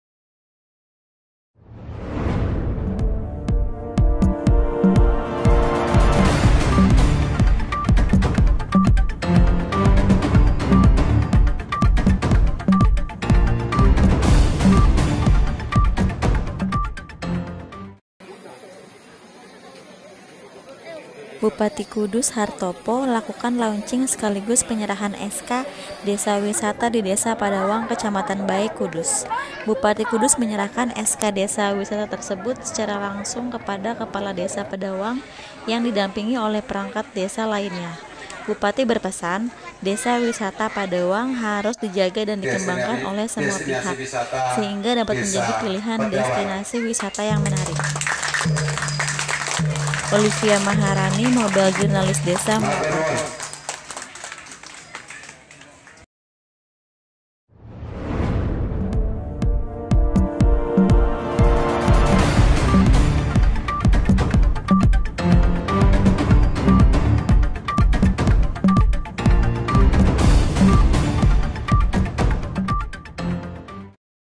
Bupati Kudus Launching Penyerahan SK Desa Wisata Padewang
Bupati Kudus, Hartopo lakukan launching sekaligus penyerahan SK Desa Wisata di Desa Pedawang, Kecamatan Bae, Kudus.

Bupati Kudus menyerahkan SK Desa Wisata tersebut secara langsung kepada Kepala Desa Pedawang yang didampingi oleh Perangkat Desa lainnya.

Bupati berpesan, Desa Wisata Padewang harus dijaga dan dikembangkan oleh semua pihak, sehingga dapat menjadi pilihan destinasi wisata yang menarik.

Olivia Maharani, Mobile Journalist Desa melaporkan